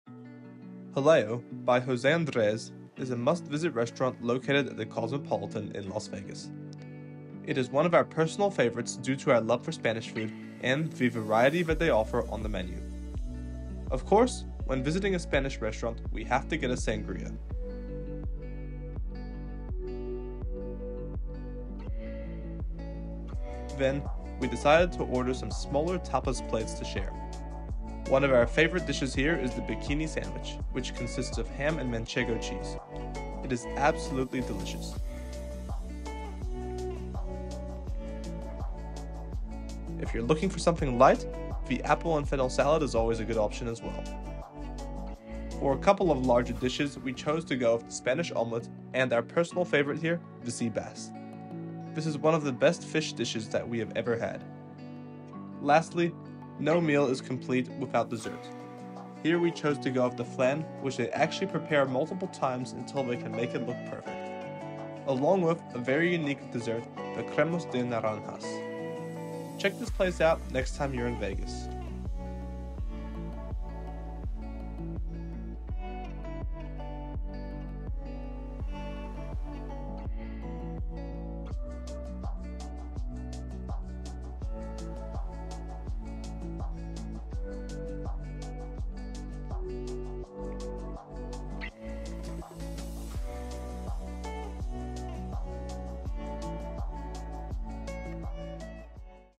Best Spanish Food on the LAS VEGAS Strip? Jaleo | GoBigOrGoHome Eats
Jaleo, by Jose Andres, is a must-visit restaurant located at the Cosmopolitan in Las Vegas. It is one of our personal favourites due to our love for Spanish food, and the variety that they offer on the menu!